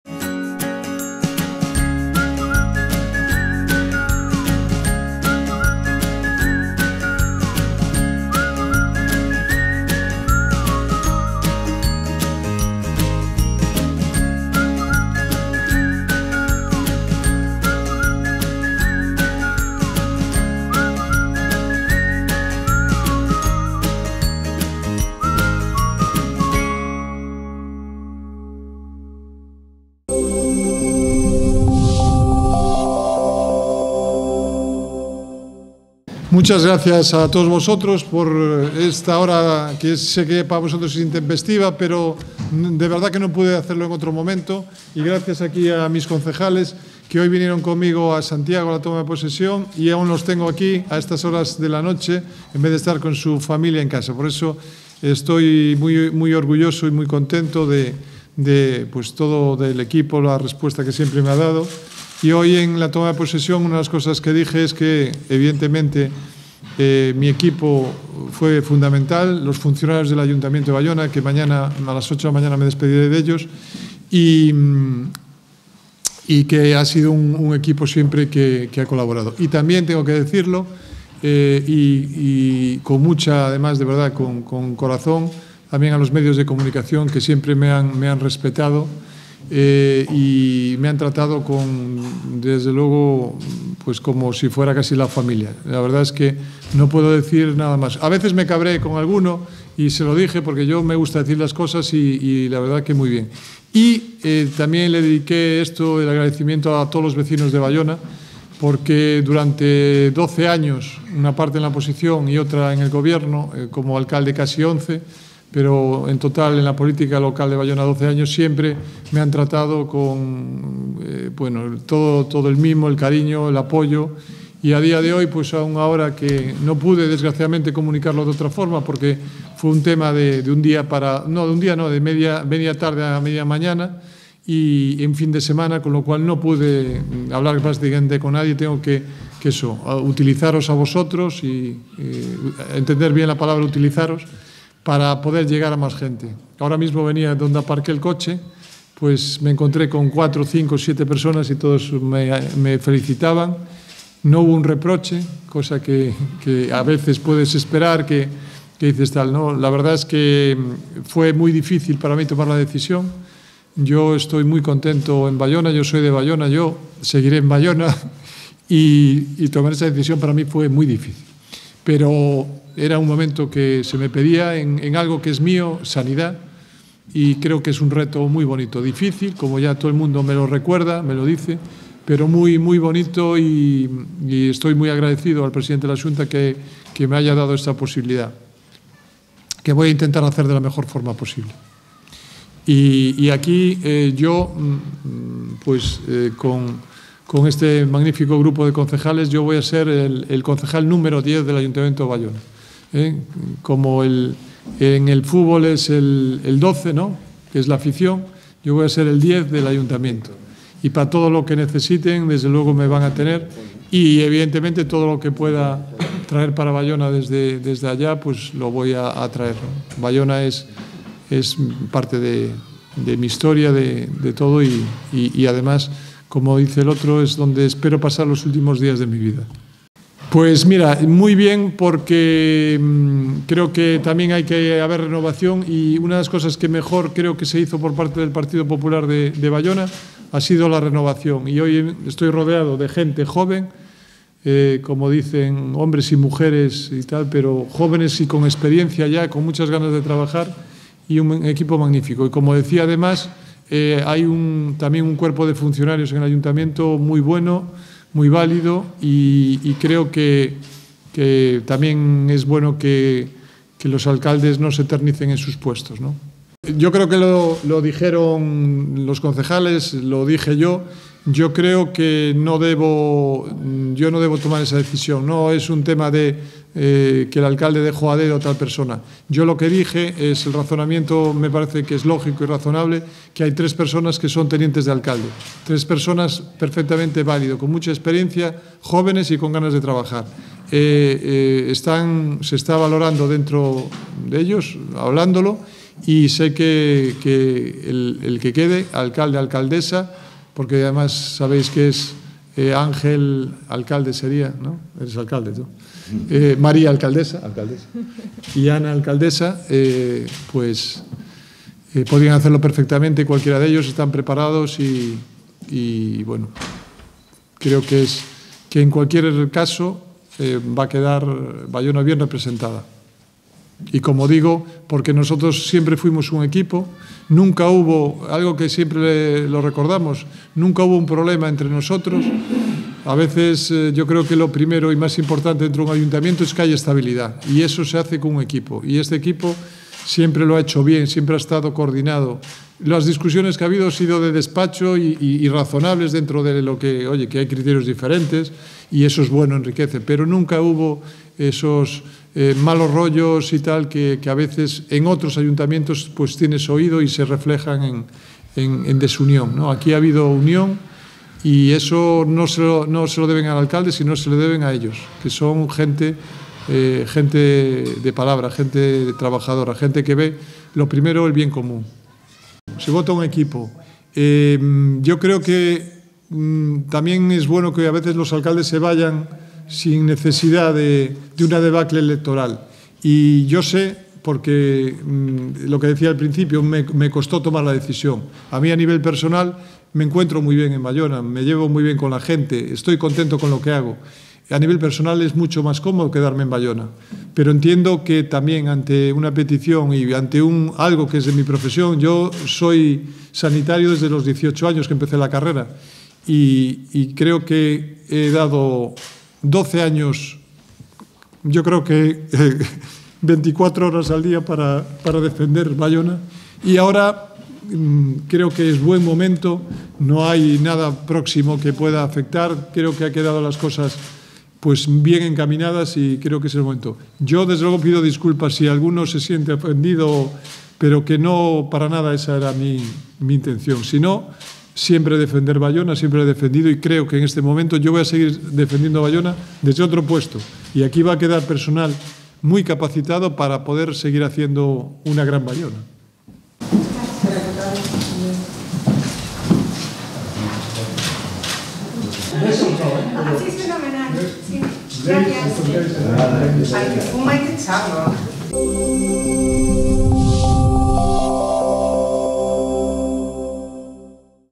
BAIONATV - Jesús Vázquez Almuíña se despide de Baiona
Rueda de prensa en la que el hasta ahora alcalde de Baiona, Jesús Vázquez Almuíña, se despide de Baiona tras su nombramiento como conselleiro de Sanidad de la Xunta de Galicia. 5 de octubre de 2015.